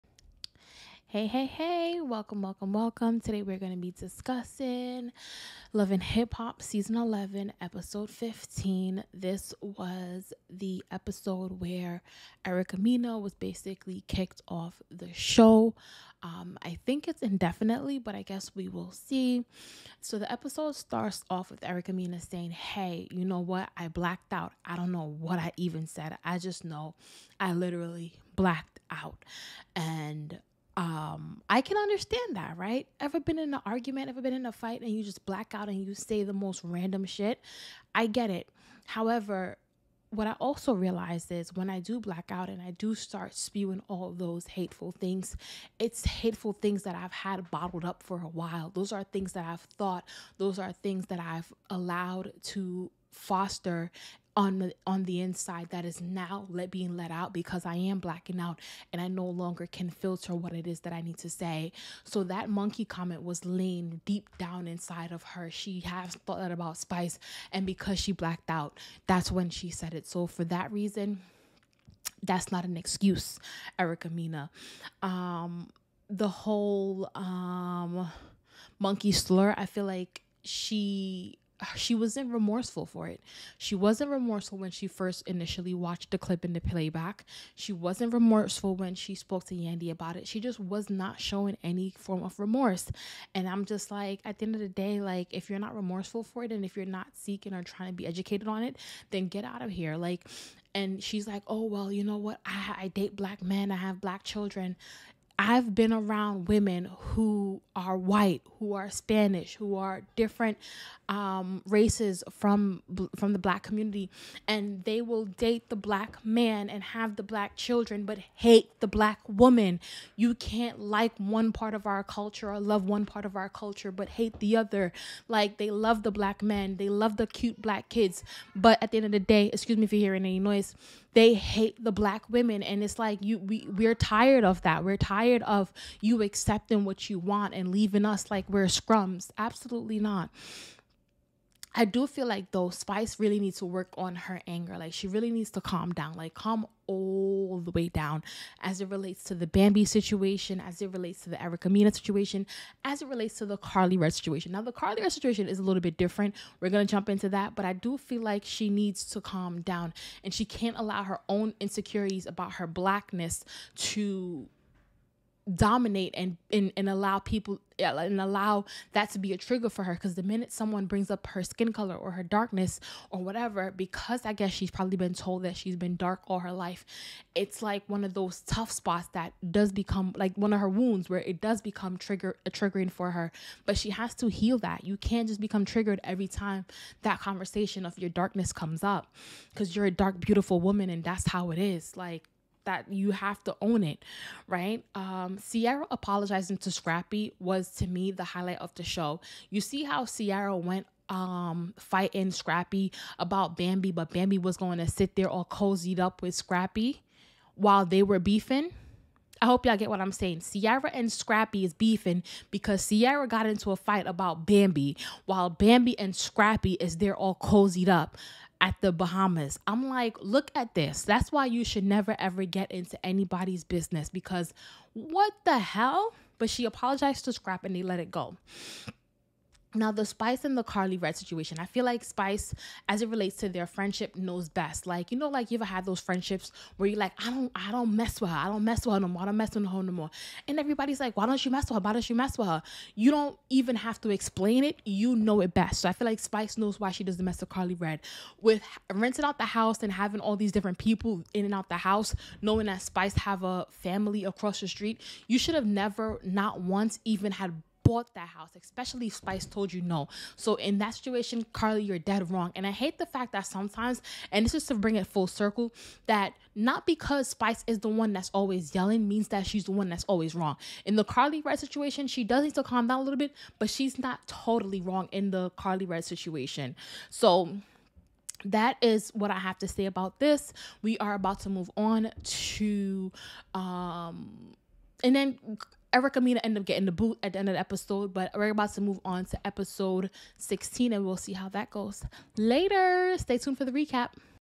Love & Hip Hop Atlanta: Season 11 Episode 15 Review - “Erica Mena Is Gone”
Welcome to Chat With Kay Ess Tee,

Today we review LHHATL episode 15.